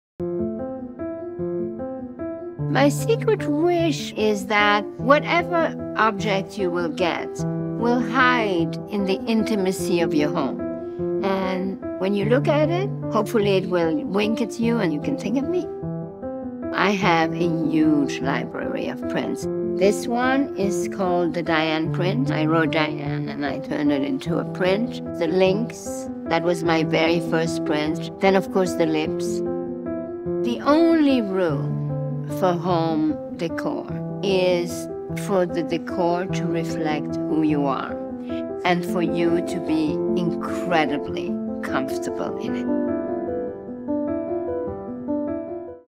Diane von Furstenberg x H&M HOME — get to know the fashion icon behind the designer collaboration
Diane von Furstenberg has got something to say — and she does it big. In this Q&A, the fashion icon tells us all about the DVF x H&M HOME designer collaboration. Get to know more about her most famous prints and patterns, and hear her give her best advise on how to decorate your home.